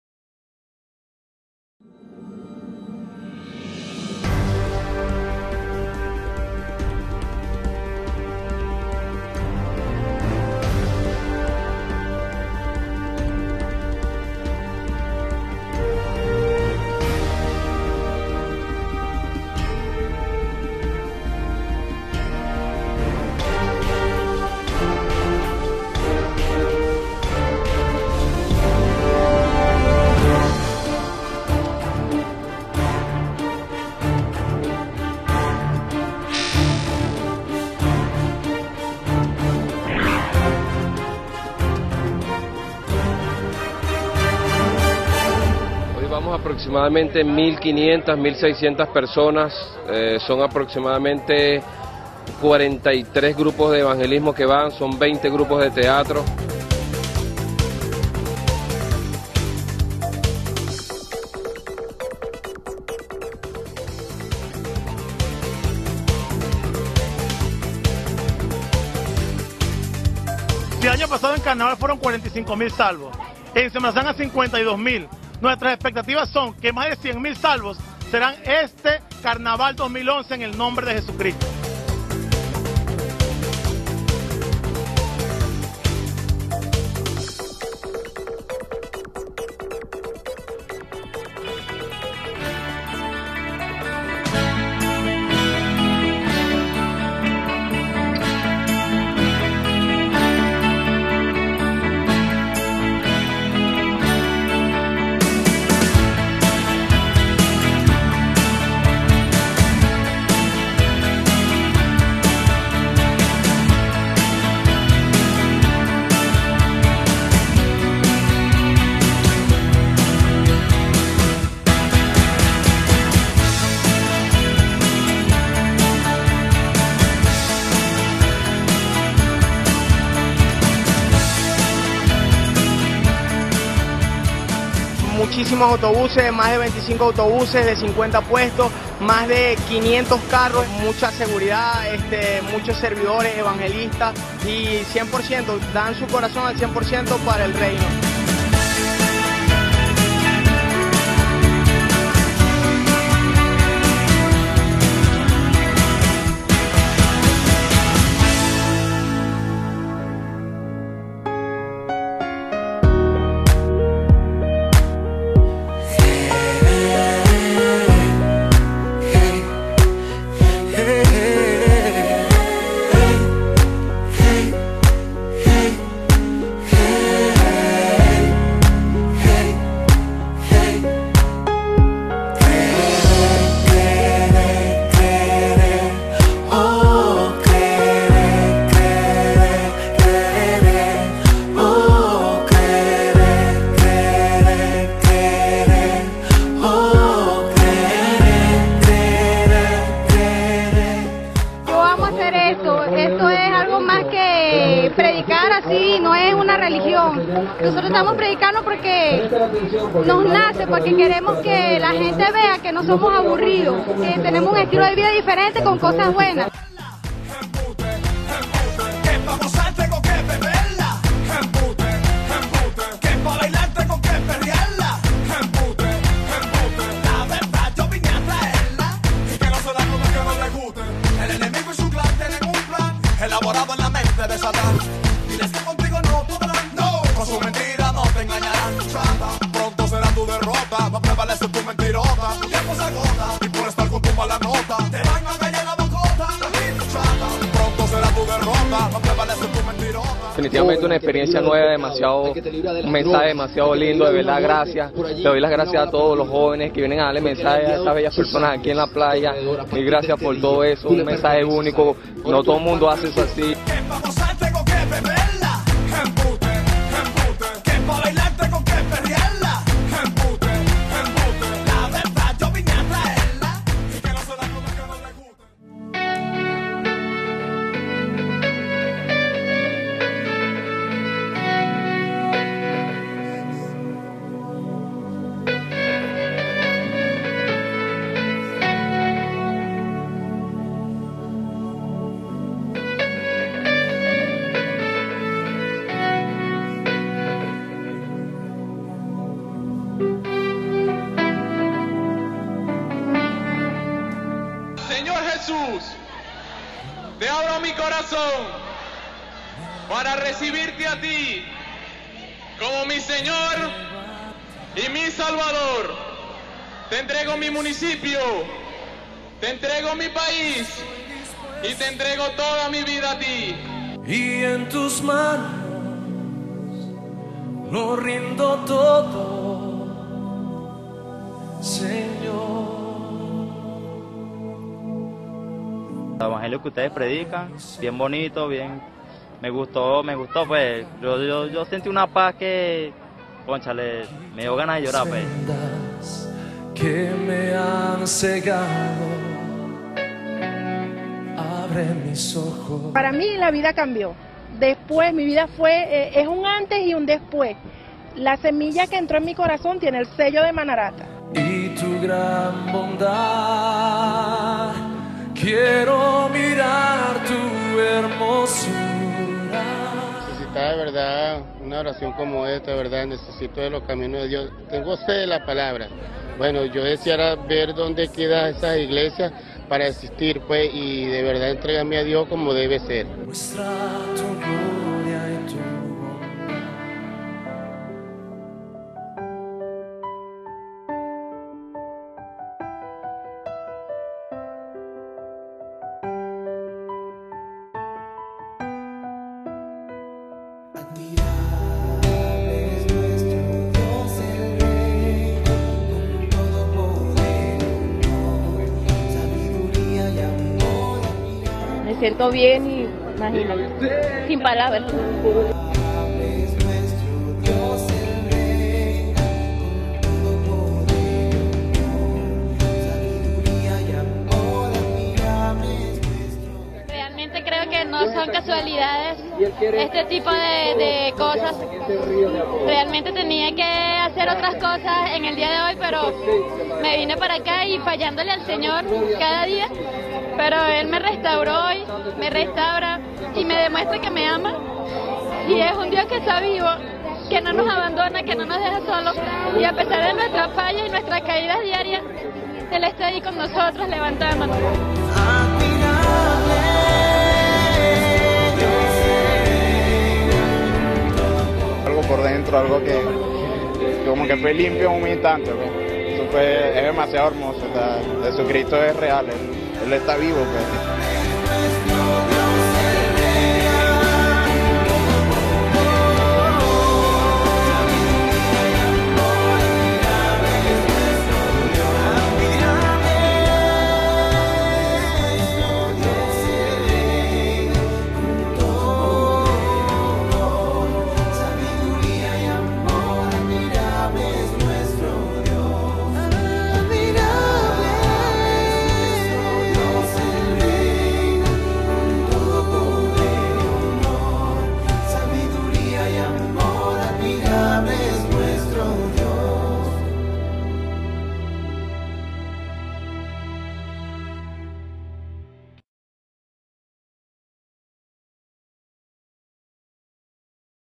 Más de 200 mil vidas salvas durante asueto carnestolendo 2011 (Lunes 07-03-2011)
Fueron más de 205 mil vidas las que fueron transformadas por el mensaje de salvación en Jesucristo, en un intenso trabajo evangelístico desplegado en playas del litoral carabobeño y falconiano por El Evangelio Cambia, el pasado lunes 7 de marzo, durante el feriado de carnaval.

De esta manera, Dios se hizo presente en el lugar para hacer sentir su amor a estas miles de personas. Se destaca la actividad especial de El Evangelio Cambia Nocturno, el cual tuvo una participación durante un concierto organizado por la Alcaldía del Municipio Puerto Cabello, en el aeropuerto Bartolomé Salom. Allí fueron 150 mil las personas que confesaron al Señor, con gran entusiasmo y gozo.

Este organizado y dinámico grupo de El Evangelio Cambia, surgió bajo la iniciativa del Pastor Javier Bertucci, con el propósito de alcanzar para Cristo a toda la nación y proclamar su verdad en todo lugar. Es así como sin límites, 20 playas fueron visitadas por los 45 grupos de evangelismo y 20 de teatro, integrados por cerca de 2 mil evangelizadores, quienes con gozo anunciaron, incansablemente, durante todo un día las buenas nuevas de salvación en Jesús.

Asimismo, cabe destacar el despliegue logístico implementado para hacer posible una actividad como esta, en los niveles de excelencia en los que se desarrolló, y que derivó en el reconocimiento de las autoridades nacionales, regionales y locales a la labor de El Evangelio Cambia.

El traslado de los evangelizadores se realizó en 35 autobuses, además de una gran cantidad de vehículos particulares, de hermanos de la congregación; todos identificados y con documentación en orden, mostrando así, la honorabilidad de la Iglesia. Asimismo, se contó con el apoyo de Protección Civil de Carabobo y Falcón, de la Guardia Nacional Bolivariana y de Tránsito Terrestre, así como de las autoridades locales, las cuales se mostraron receptivas a todo el trabajo social desarrollado por la Iglesia Maranatha Valencia. En cuanto a la seguridad, cada playa contó con un personal responsable en la materia, así como cada autobús y todo carro identificado con la actividad.

Finalmente, la vida de muchos fue sorprendida por el amor de Dios en un espacio como este, viéndose vidas quebrantadas rindiéndose ante la presencia de Dios y acercándose a la fuente de la vida, Jesucristo. El Evangelio Cambia estará presente nuevamente en las playas de Carabobo y Falcón durante el venidero asueto de Semana Santa, para extender el Reino de Dios.